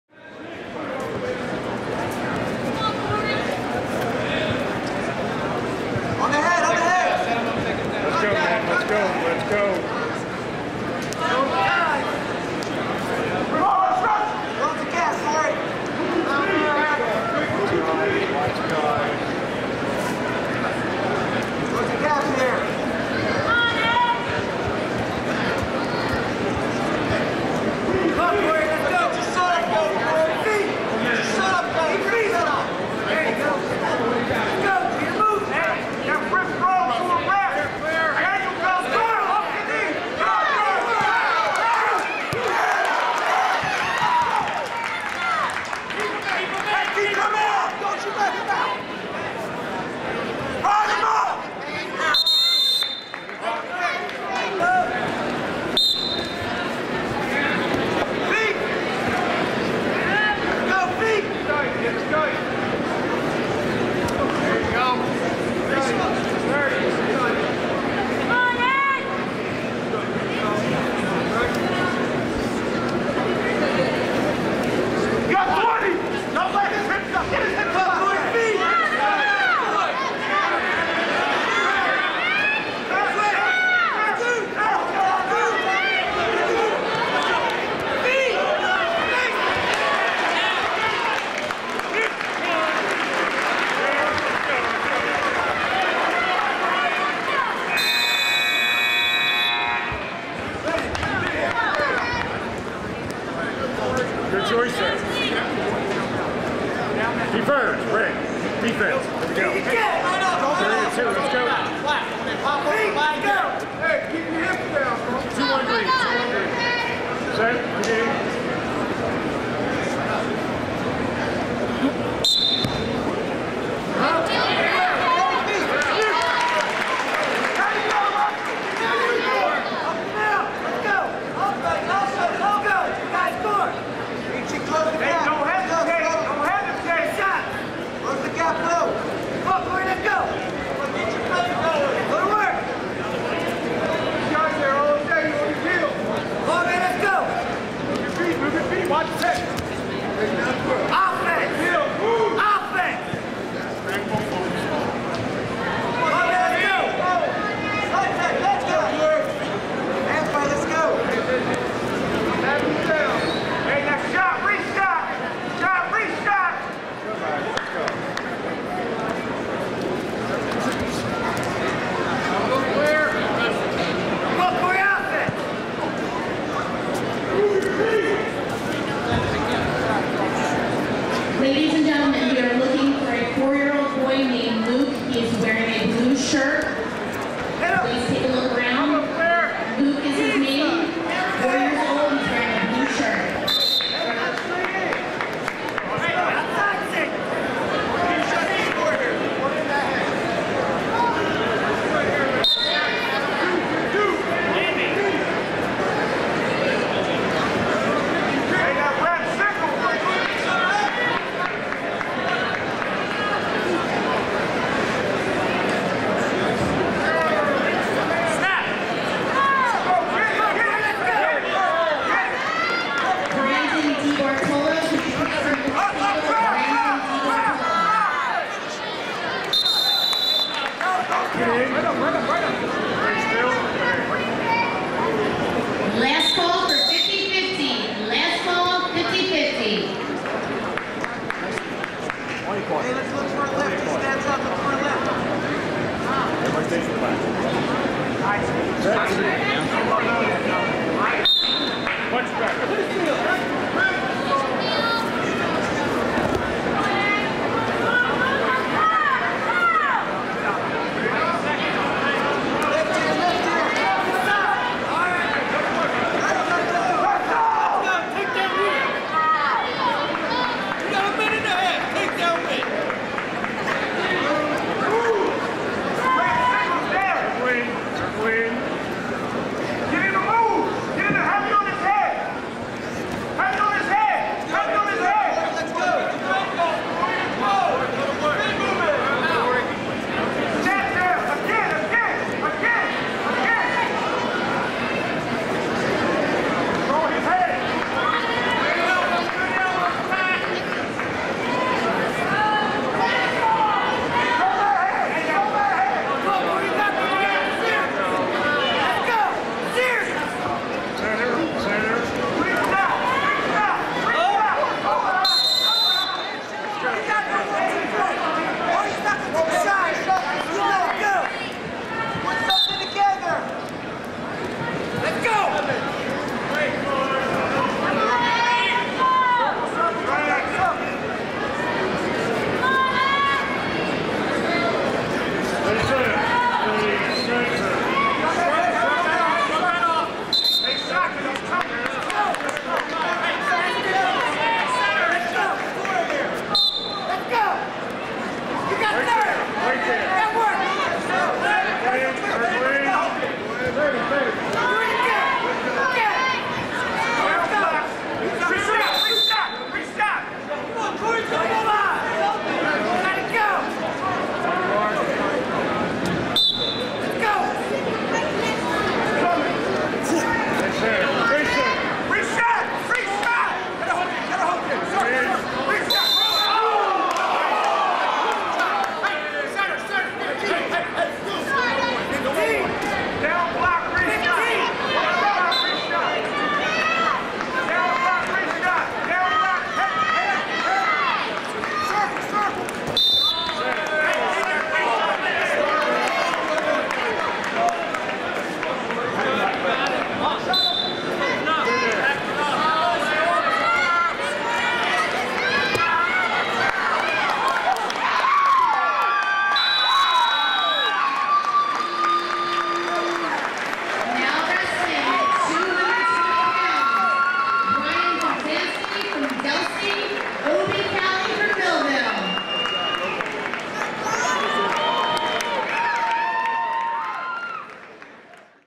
Region 8 Wrestling Finals: Ed Shockley beats top ranked Cory Damiana at 195
In a battle of two undefeated wrestlers, Ed Shockley of Millville edged out Cory Damiana of Lower Cape May, 3-2, in the 195-pound Region 8 final at Egg Harbor Twp. High School. Damiana was The Star-Ledger&apos;s top-ranked 195-pounder in the state.